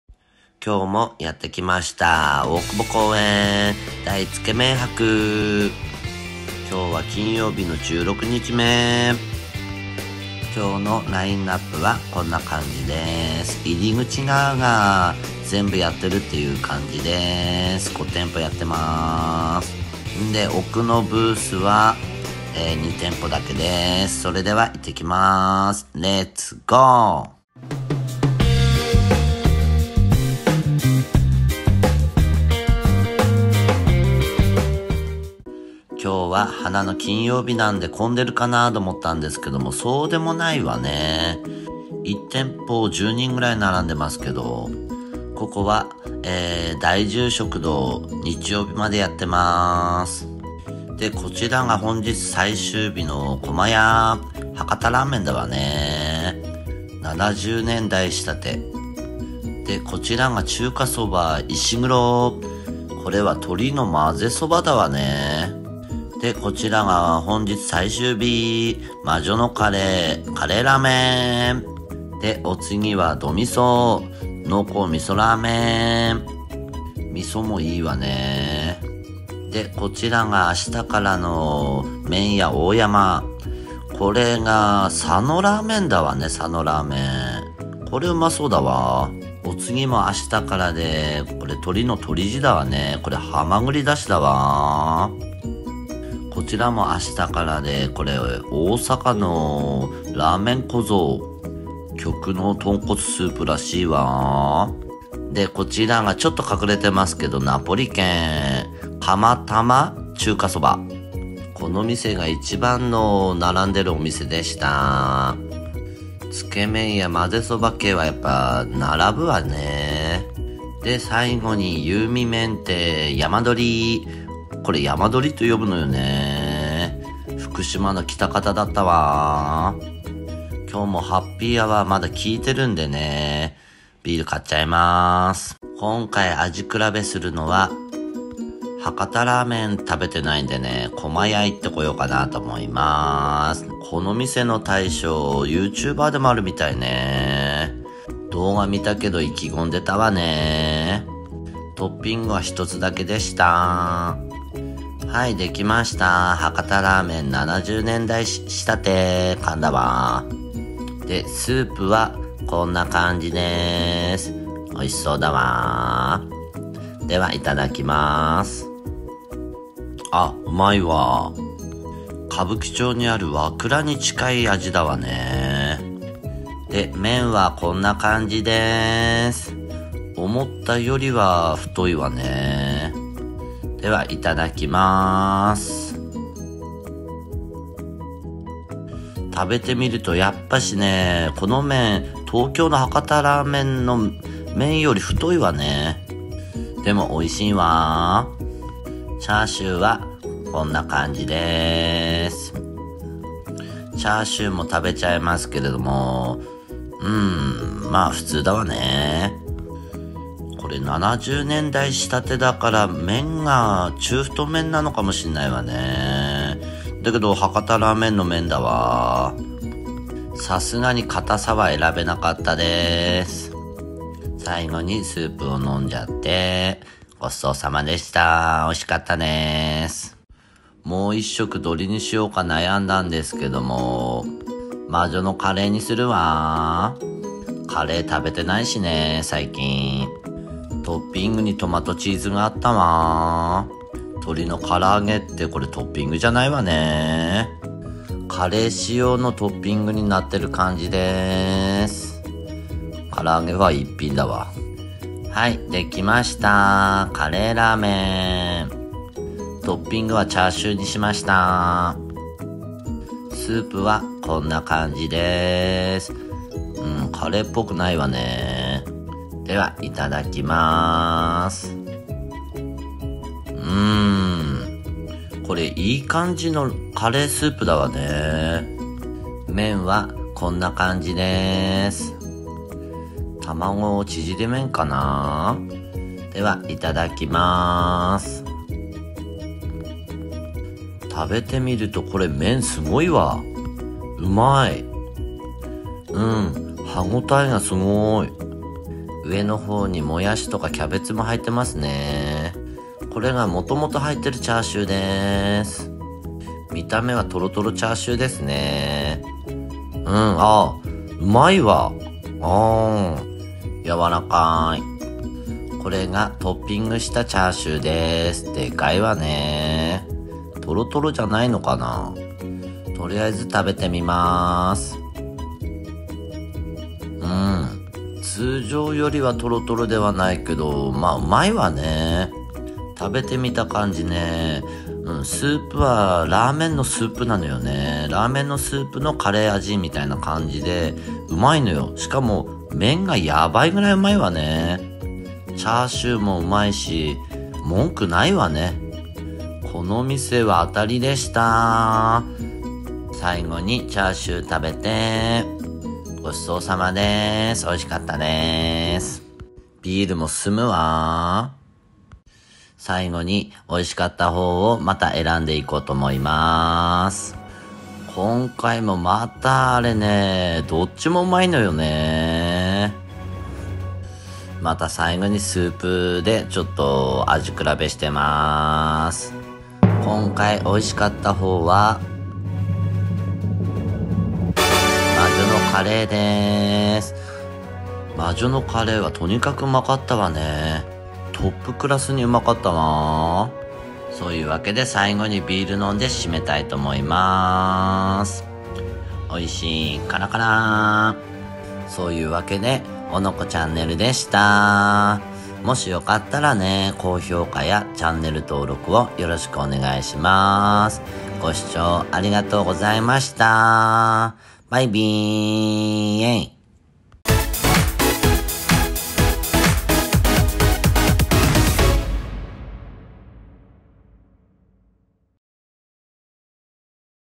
大久保公園！大つけ麺博！世界一美味いラーメン祭！16日目
大久保公園で開かれてる、大つけ麺博 世界一美味いラーメン祭に行きました。

三年ぶりに開催されたつけ麺博は、今回から現金で買えなくなり電子マネーのみとなりましたので、ご注意下さい。
Suica、PASMO、ナナコなどは使えますが、PayPayはドリンク類しか使えませんでした。
50mくらい先にセブンイレブンがあるので、そこでチャージをすると良いと思われます。

平日11:00～18:00までハッピーアワーでドリンク類が一律３００円です。

大つけ麺博 世界一美味いラーメン祭の動画は、特別枠で金曜日、日曜日の両方、又はどちらかで配信します。
火曜日は通常の動画です。

［動画目次］
00:00 本日のラインナップ、オープニング
00:36 会場内紹介
02:16 駒や
03:50 魔女のカレー
06:02 締め、エンディング